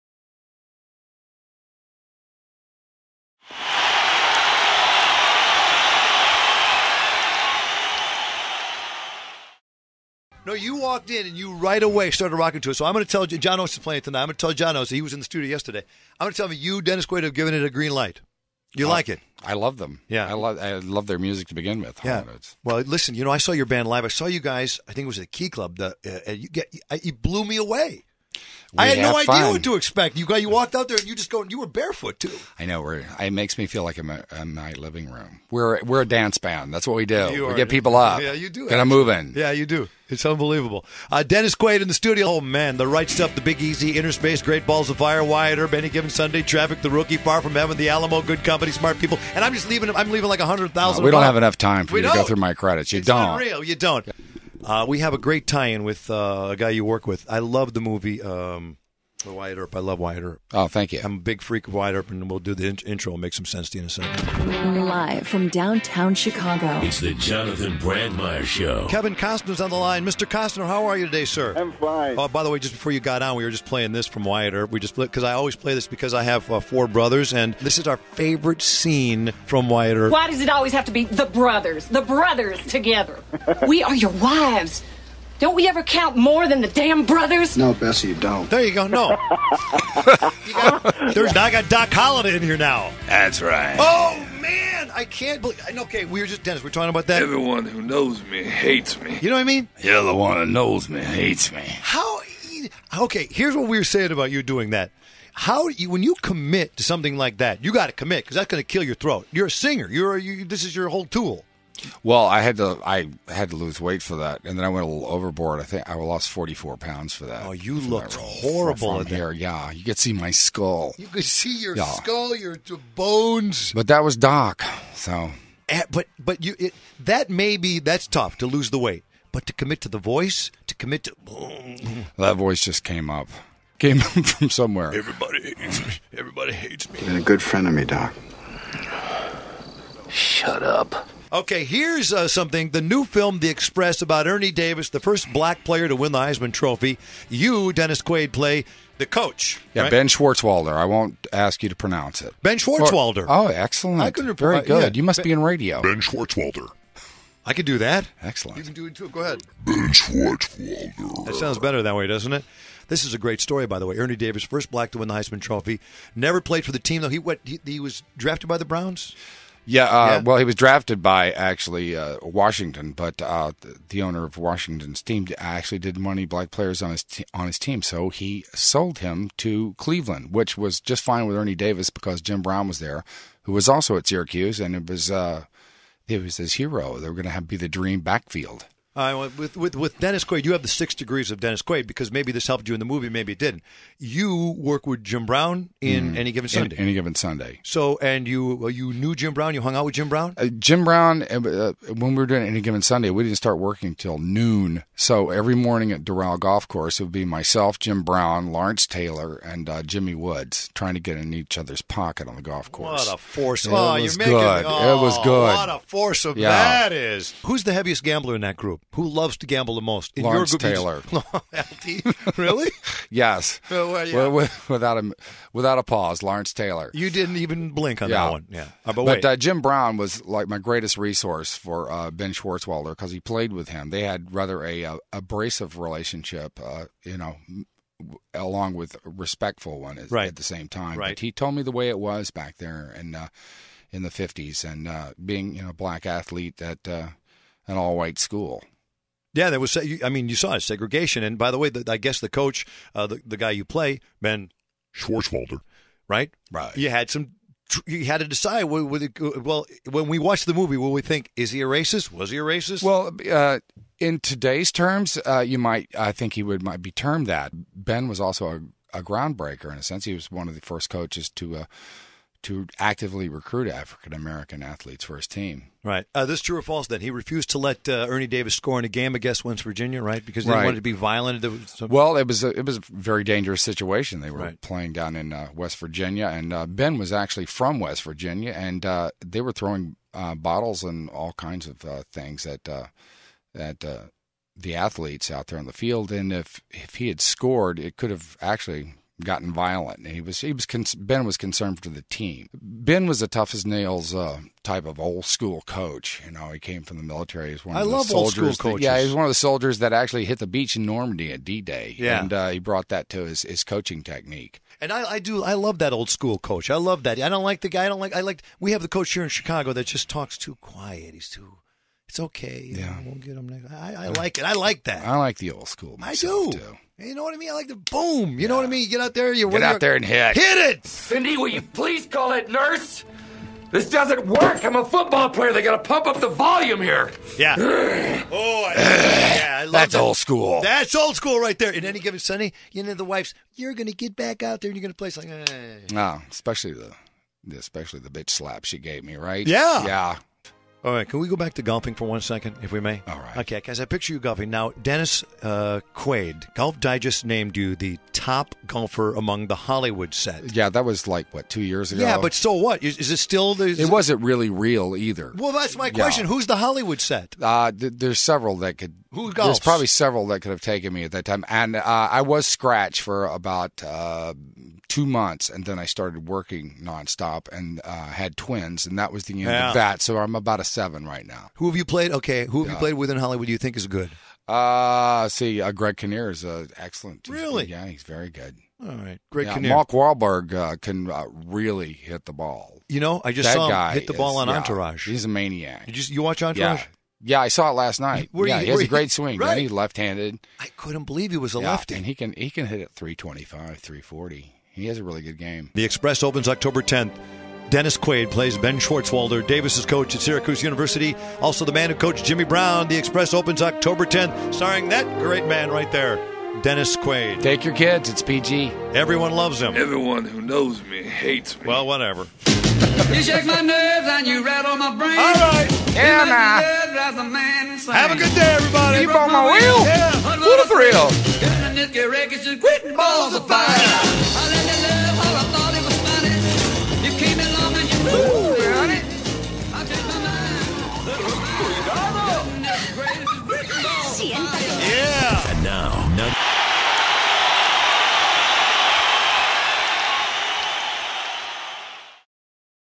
Dennis Quaid on JB!
Dennis Quaid discusses his new film and past roles!